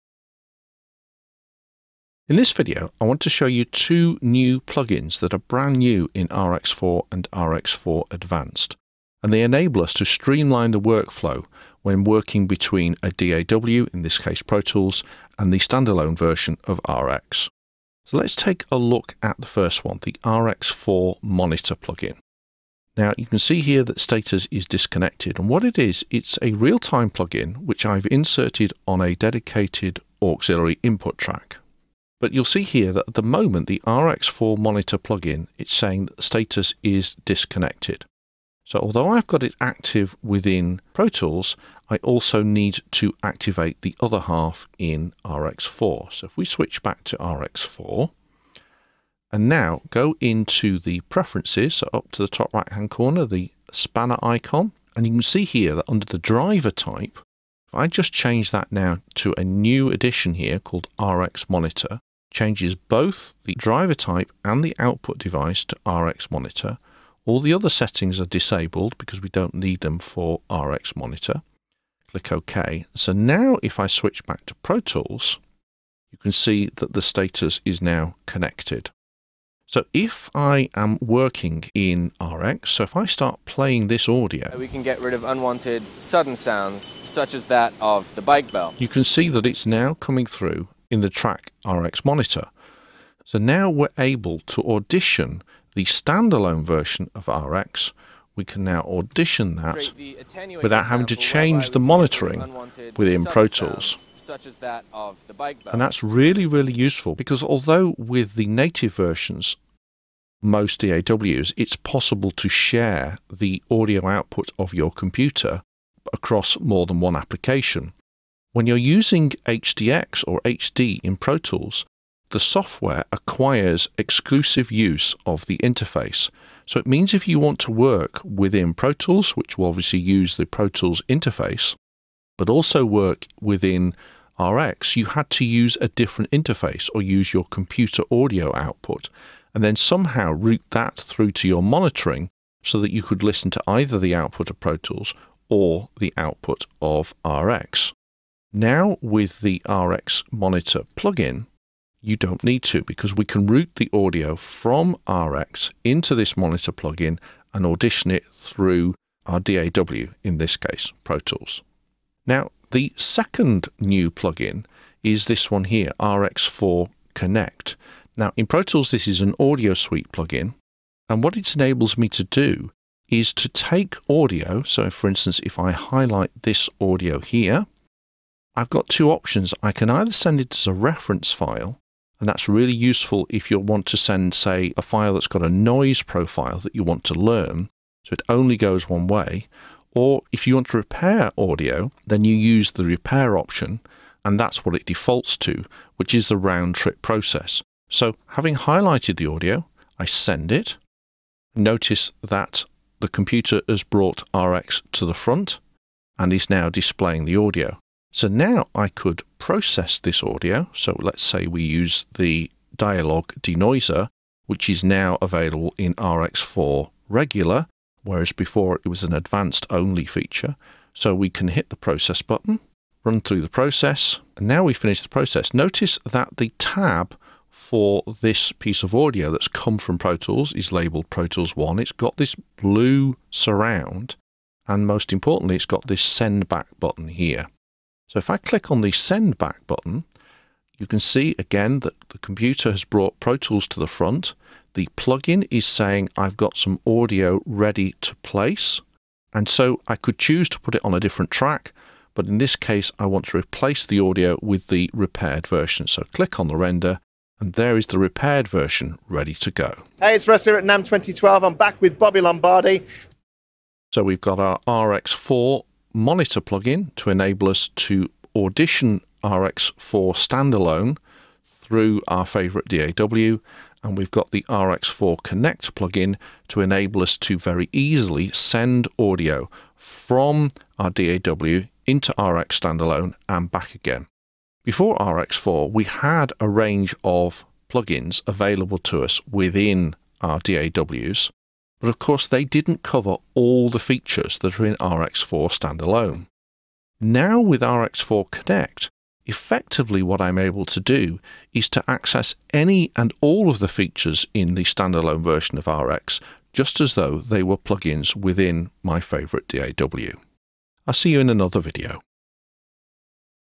RX Monitor & RX Connect Plug-Ins (iZotope RX 4 Explained)
In this video Mike shows how you can use the new RX 4 plug-in to monitor the standalone RX from within your DAW and you can get the best of both worlds with the new RX 4 Connect plug-in to export audio from your DAW out to RX 4, work on it in the standalone app and then bring it back into your DAW.